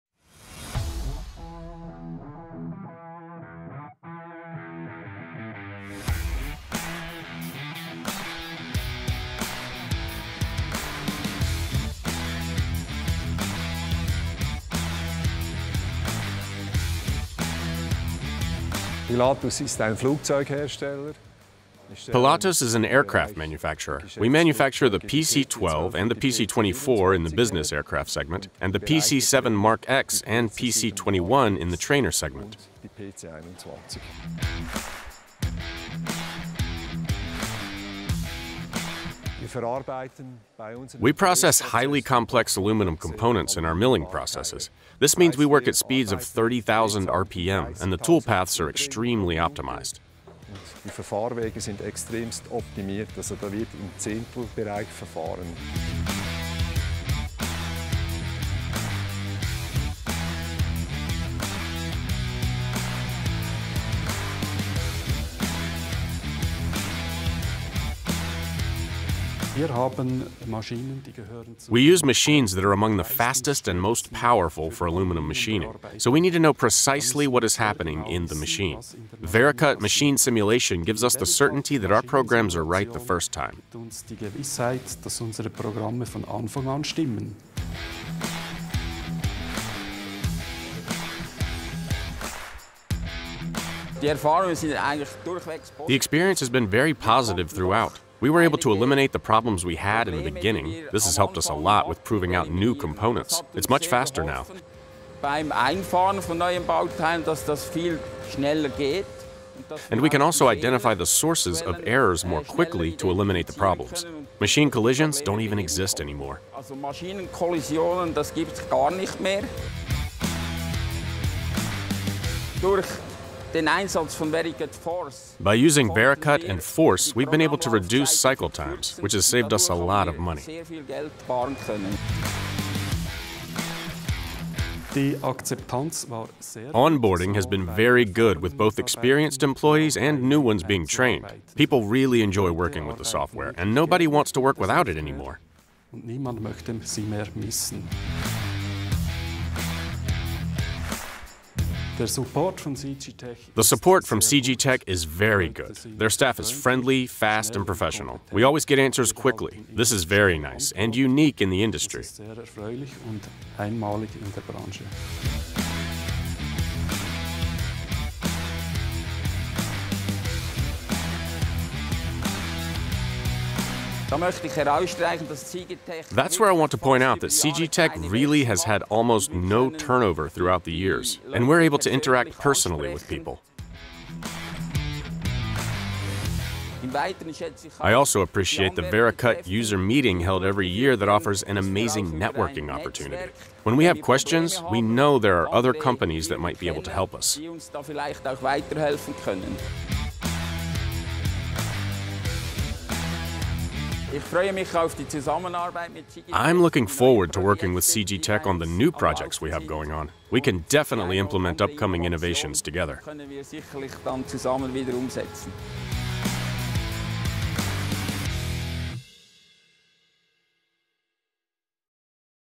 Pilatus - Error-Free High-Speed Machining | VERICUT Testimonial
Global aerospace manufacturer Pilatus uses VERICUT to simulate NC programs and prevent costly crashes on powerful machines.